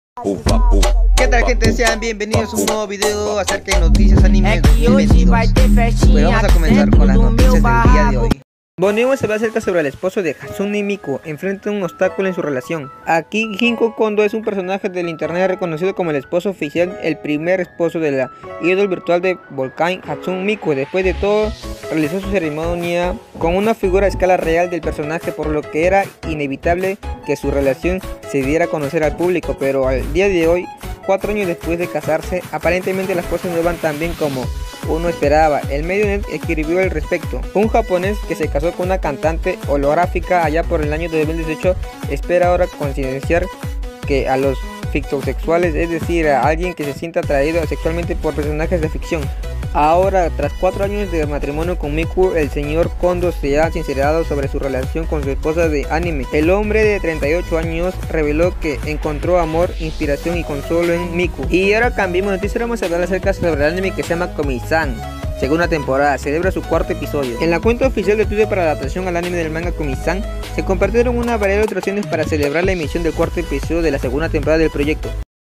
El ‘esposo’ de Hatsune Miku enfrenta un obstáculo en su relación
Akihiko Kondo es todo un personaje de internet, reconocido como “El Esposo Oficial / El Primer Esposo” de la idol virtual de Vocaloid, Hatsune Miku. Después de todo, realizó su ceremonia nupcial con una figura a escala real del personaje, por lo que era inevitable que su “relación” se diera a conocer al público. Pero al día de hoy, cuatro años después de casarse, aparentemente las cosas no van tan bien como uno esperaría.